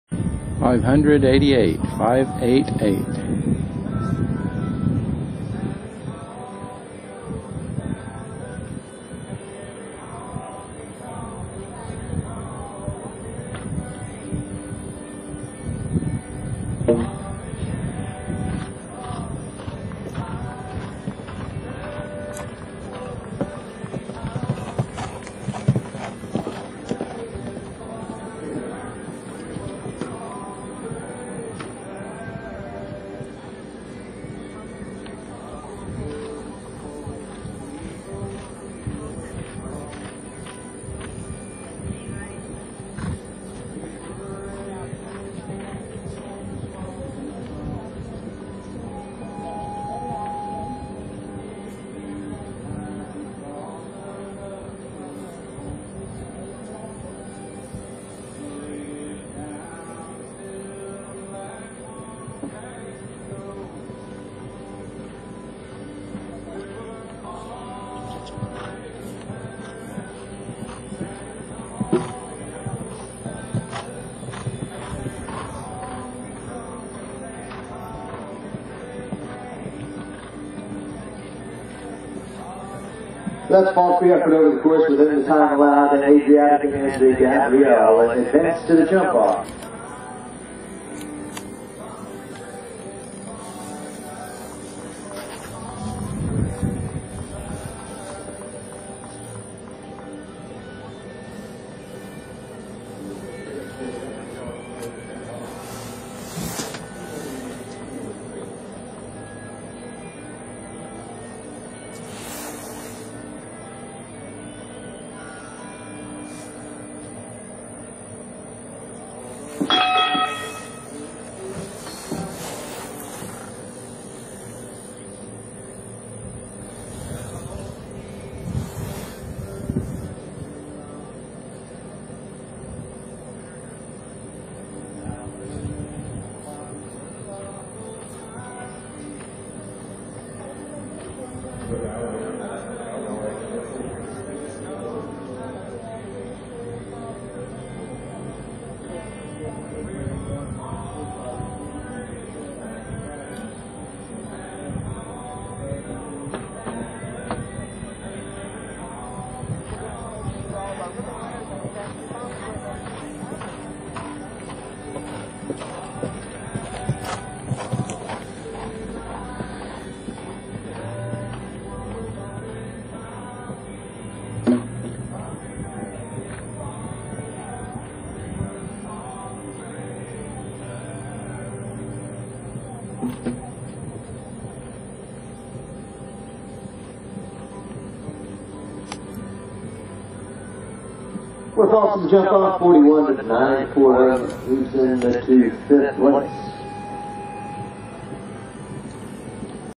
Video of ADRIATICA ridden by GABRIELLE LEBOEUF from ShowNet!
ADRIATICA ridden by GABRIELLE LEBOEUF at Gulf Coast IV.  This round is from class 533, NAL Jr AM Jpr Classic 1.25m II2ab and finished 11 of 33.